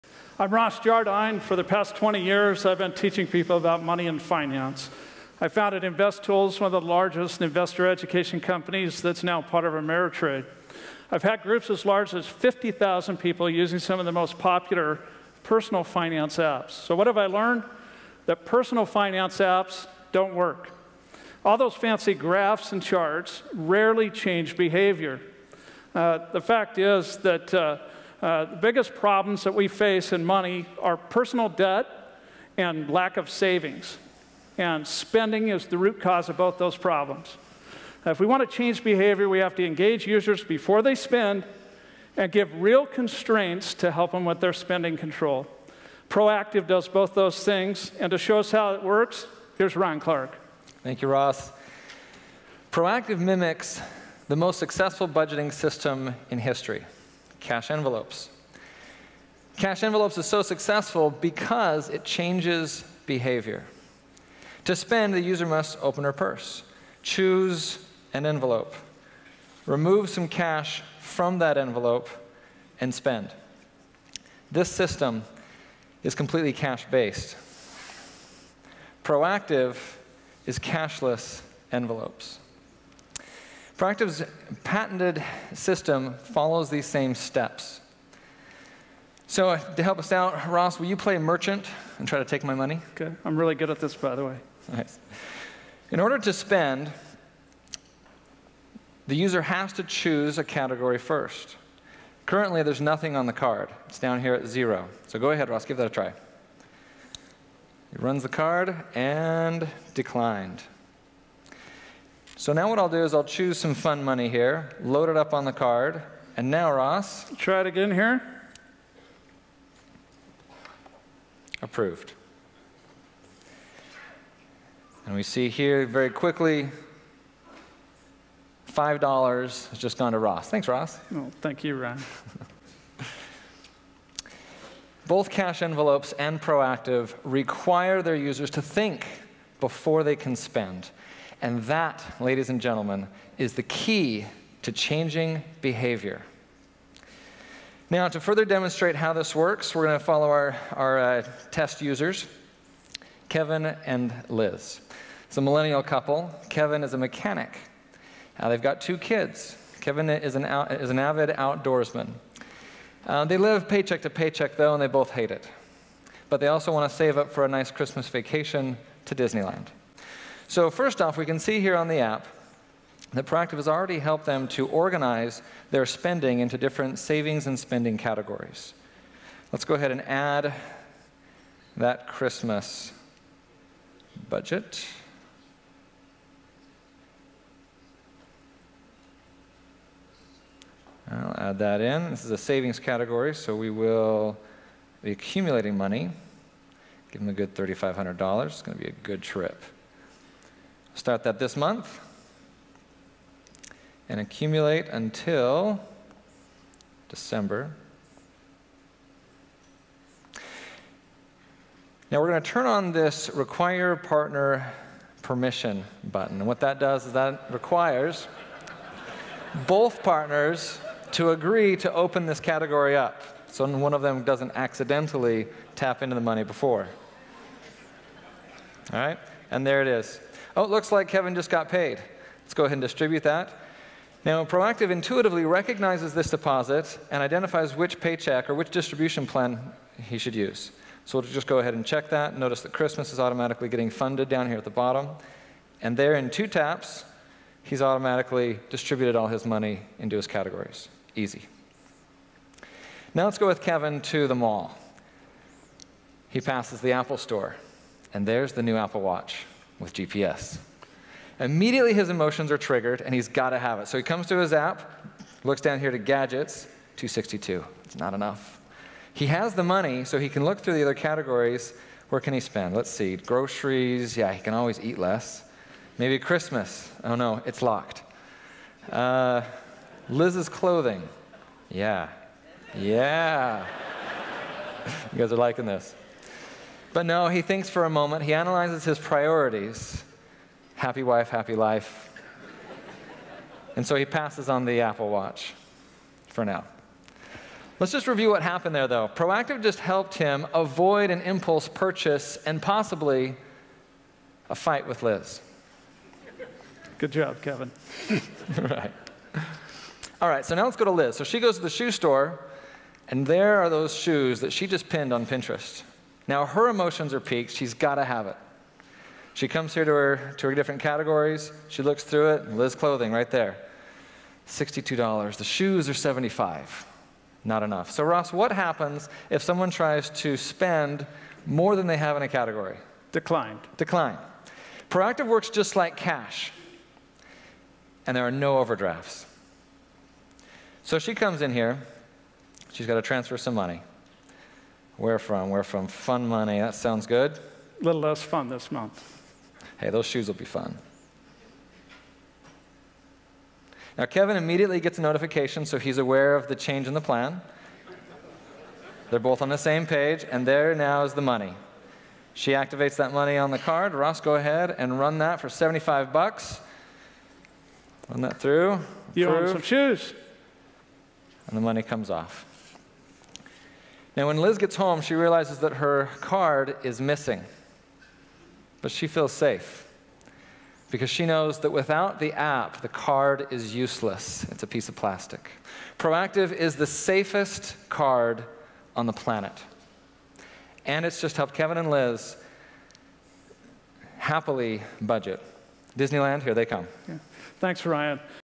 Cash Envelope Budgeting - NOW DIGITAL - ProActive Live Demo
iPhone only.  Android is coming!

This is just like Dave Ramsey's "Envelope Budget" but with a debit card.  Shop online, Share with your spouse, No Overdrafts, No Pending Transactions.  ProActive is Real Time!  Tap, Swipe, and you're done! Budgeting as easy as Spending.